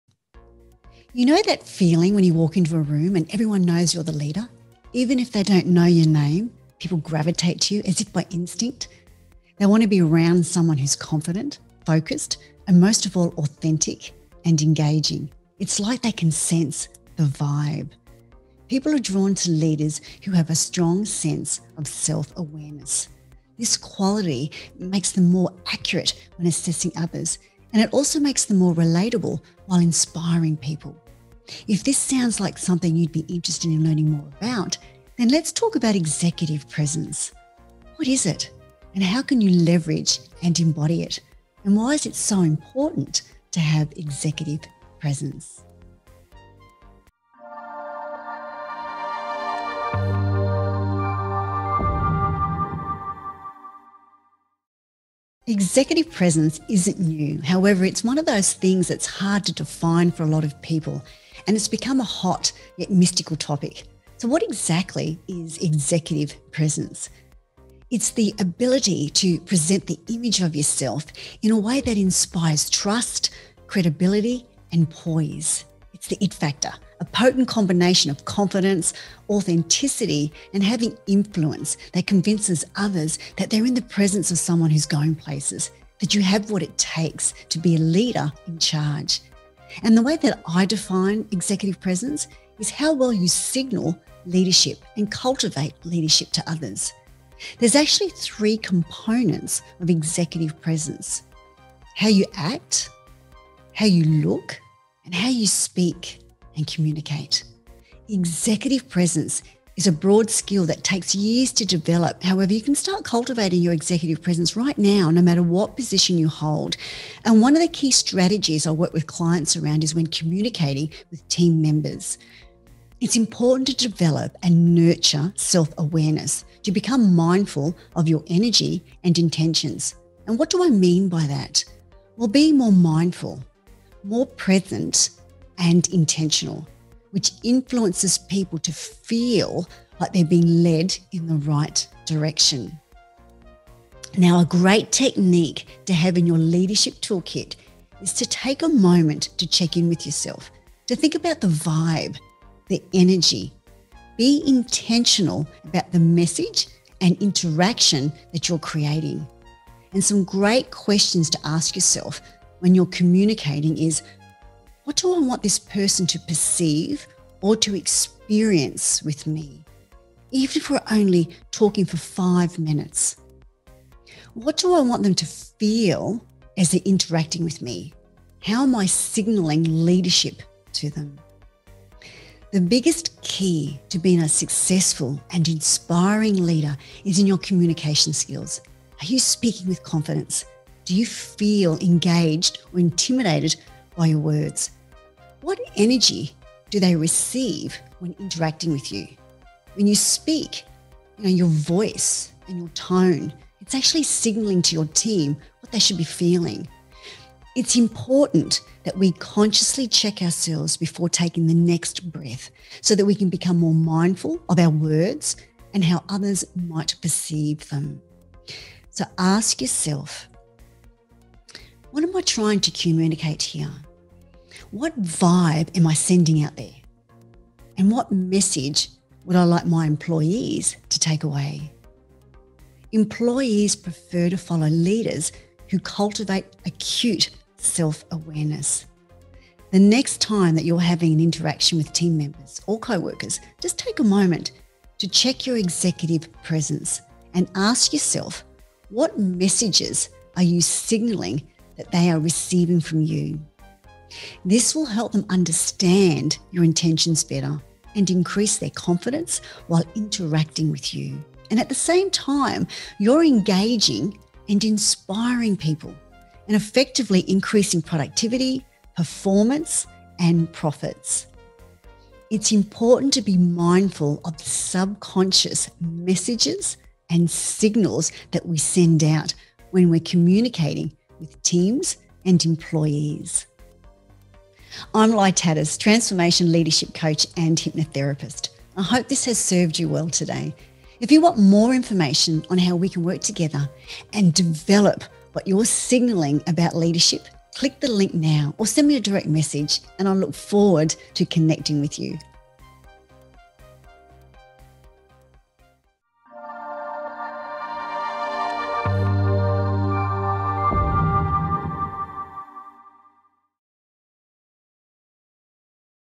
Executive Presence - What's The Secret Sauce?
Executive presence is what makes others take notice when you walk in the room.

In this video we'll talk about how it works and how you  can signal your Executive Prescence to make an impact andinstantly attract intense, positive attention.